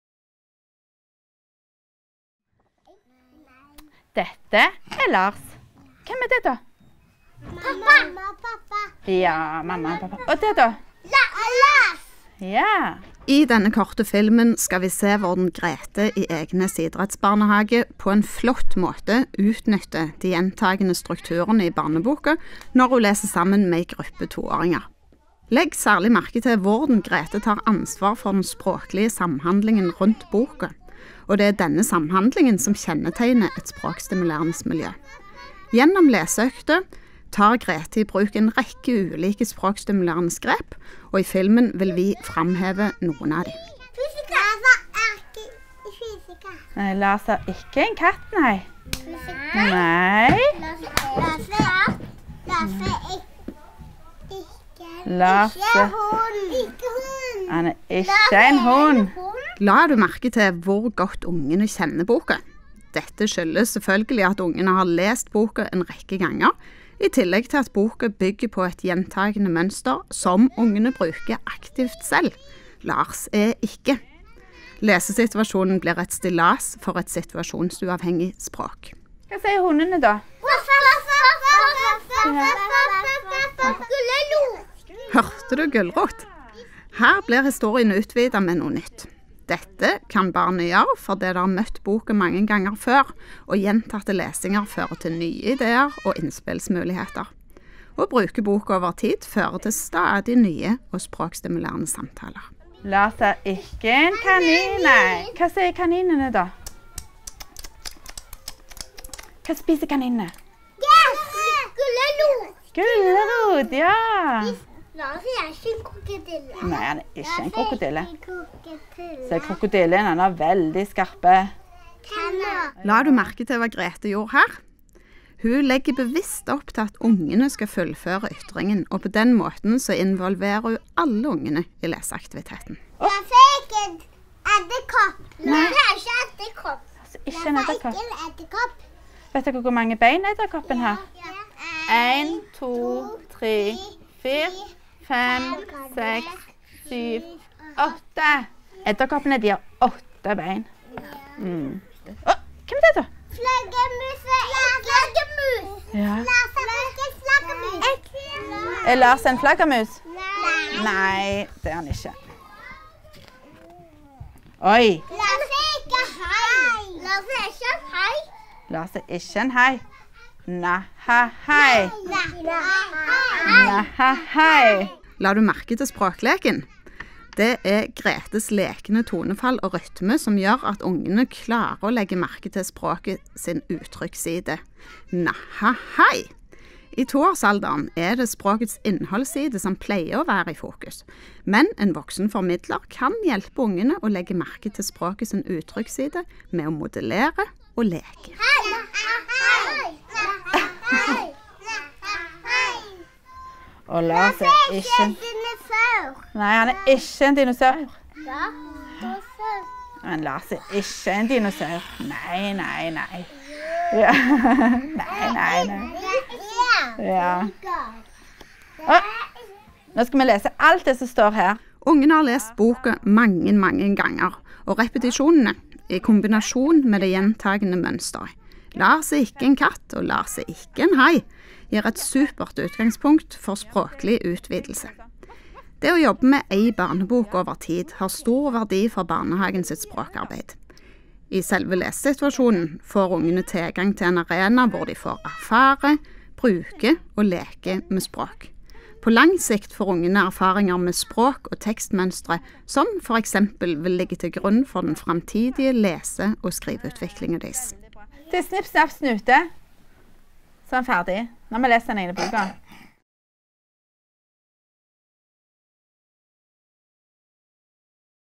Å lese bøker med gjentakende mønster
Det å lese bøker med gjentakende mønster kan være en god innfallsvinkel i barnehagens språkarbeid. I denne filmen kan du se eksempel på hvordan dette kan gjøres.